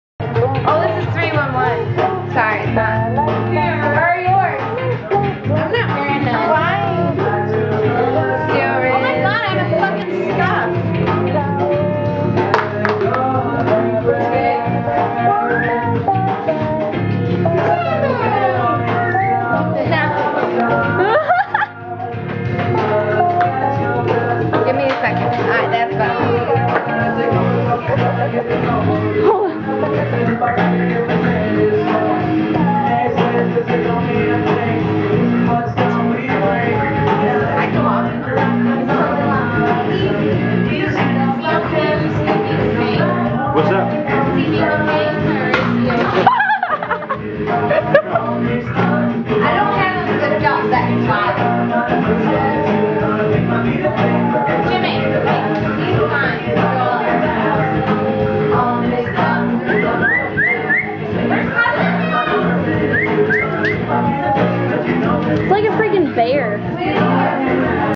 SCUFFED SHOE
pissed i just seen a scuff on my heels